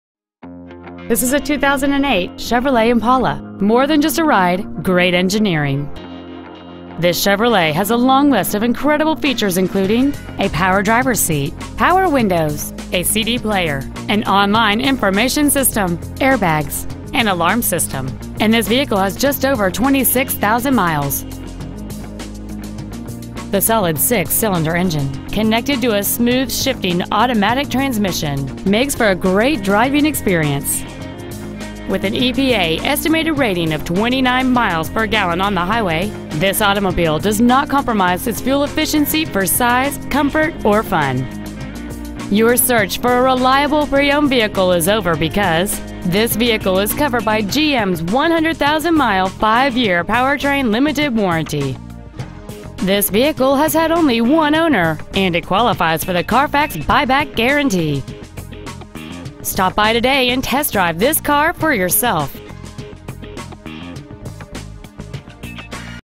Used 2008 Chevrolet Impala Irving TX 75062
Year: 2008
 Make: Chevrolet
 Model: Impala
 Engine: 3.5L V-6cyl
 Trans.: 4-Speed Automatic
 Exterior: Red
 Miles: 26,965
 Interior: Gray